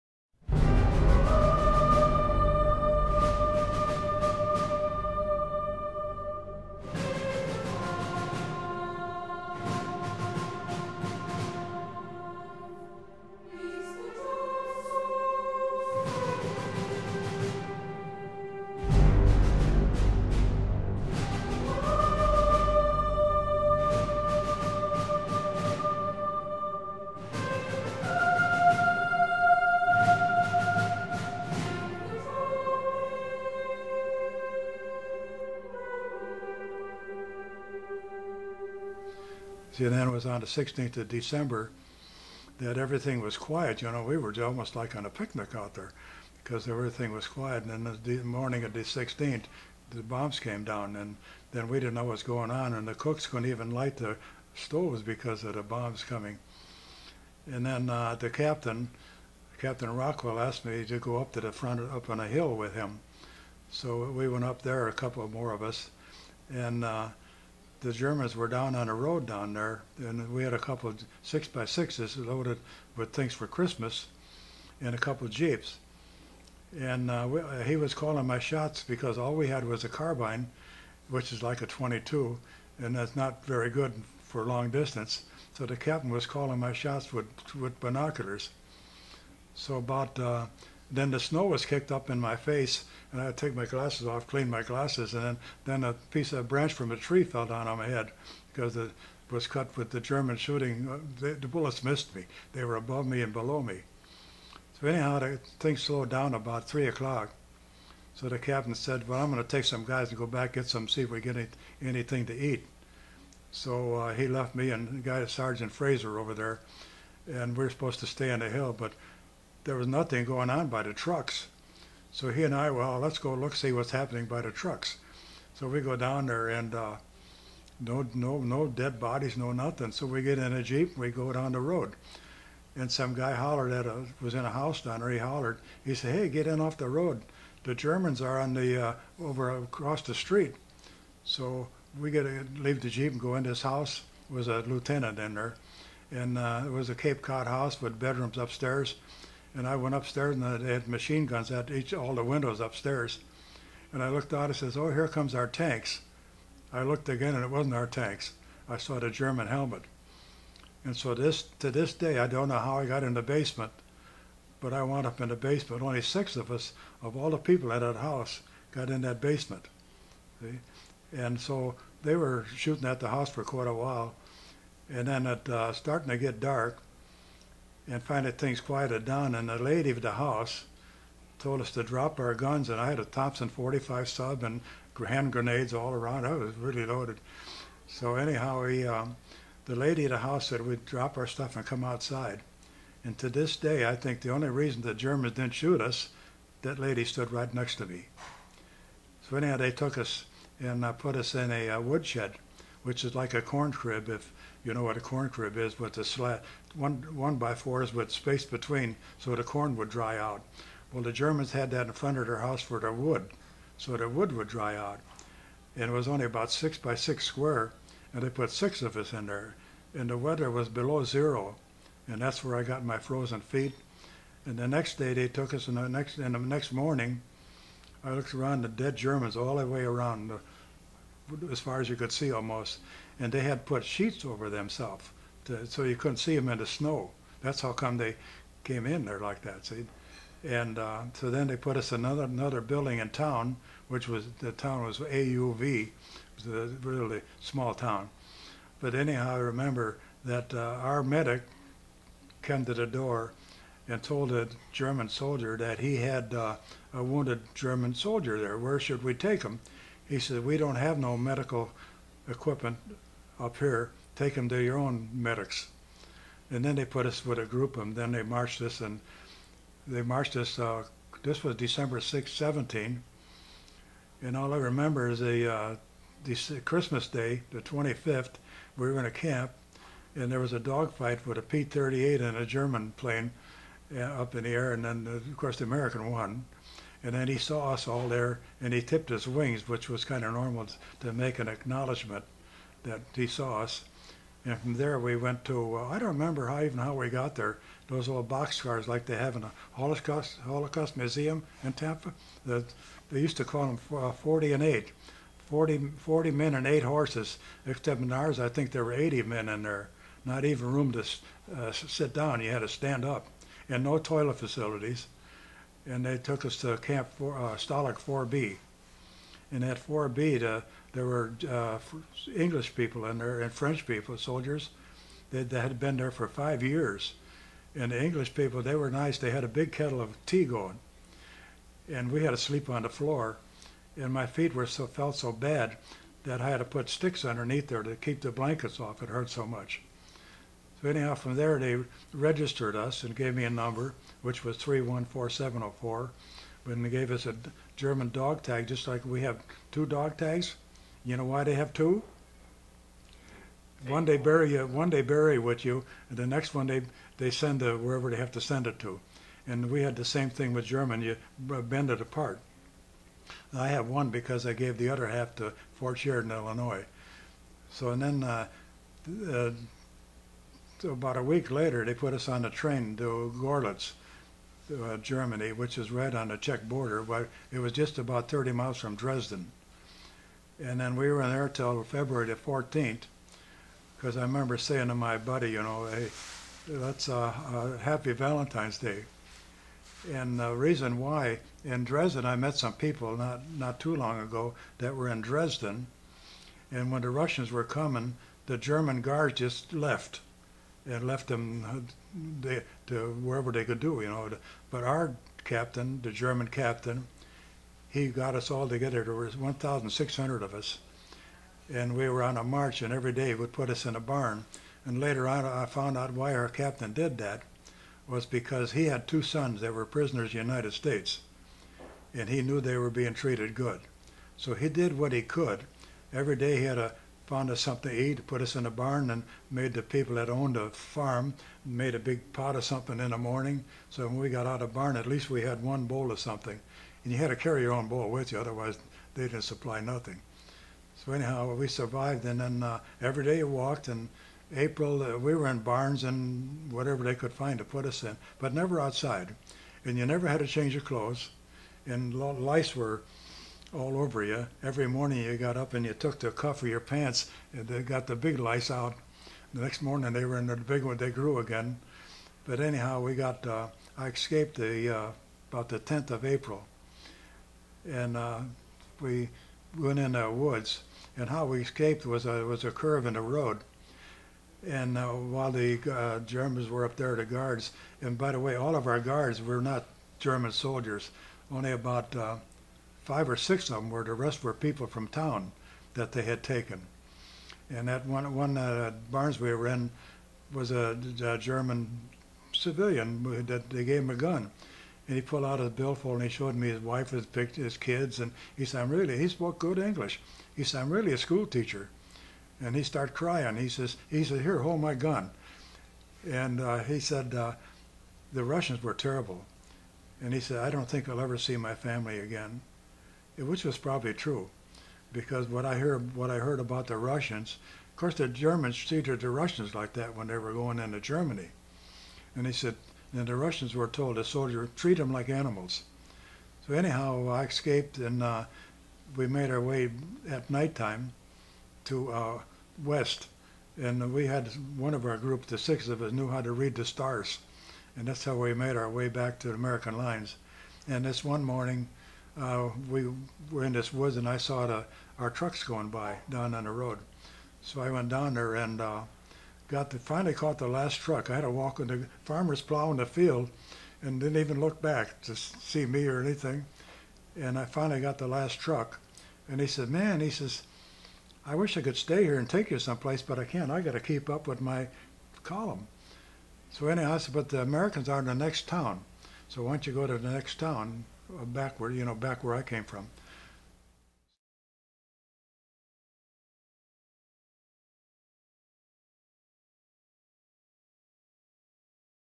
US Army WW2 Veteran Recalls Being Captured by German Soldiers During the Battle of the Bulge
T/4 John C Terborg describes his encounter with German troops on December 16, 1944, the start of the Battle of the Bulge.  Being directly on the frontlines, John is immediately captured by advancing German soldiers and sent behind the lines into captivity.  He summarizes his experience as a P.O.W. until his escape on April 10, 1945.